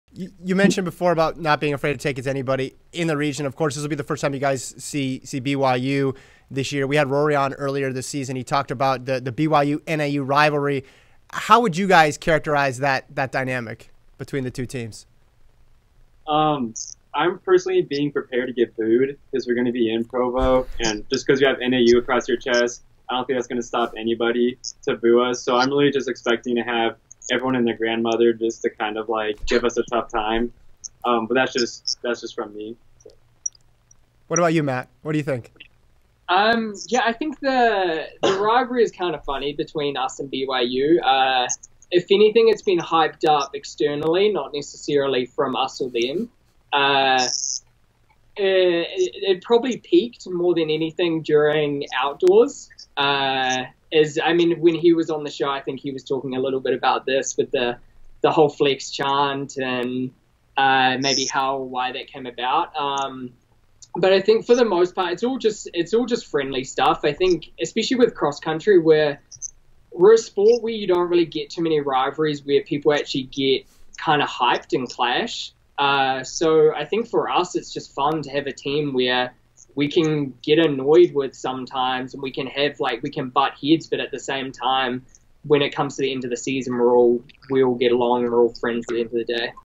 Tyler Day Is Prepared To Get Booed At Regionals
Before they head to Provo for the Mountain region meet, Day and Matt Baxter give their thoughts on the NAU/BYU rivalry.